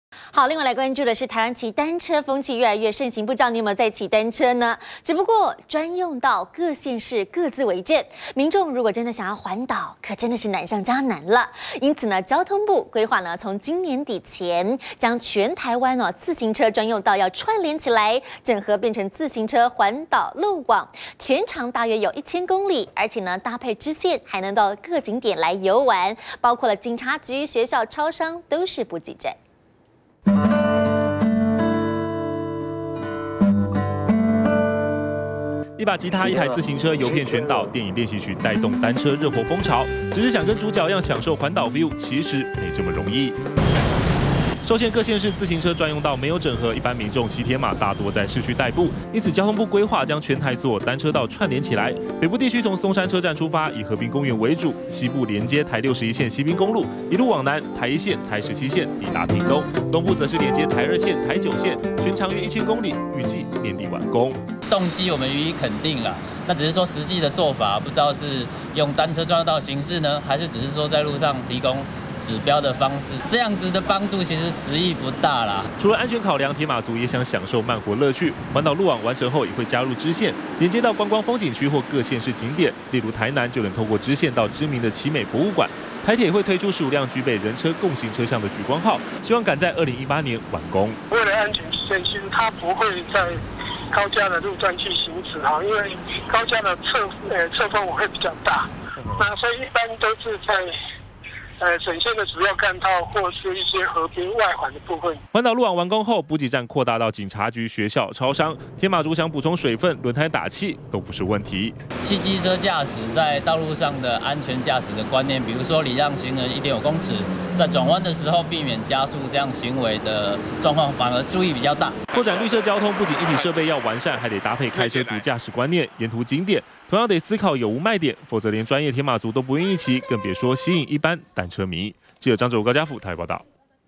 20150118中天新聞 專用道大串聯 「單車國道」年底上路
台灣騎單車風氣越來越盛行，只是專用道各縣市各自為政，民眾想環島難上加難，因此交通部規劃今年底前，將全台自行車專用道串聯起來，整合成自行車環島路網，全長約1000公里，搭配支線還能到各景點遊玩，警察局 學校 超商，都是補給站，但除了硬體設備外，對鐵馬族來說，最重要的還是沿途點，以及開車族的駕駛觀念，才是能不能吸引的主要原因。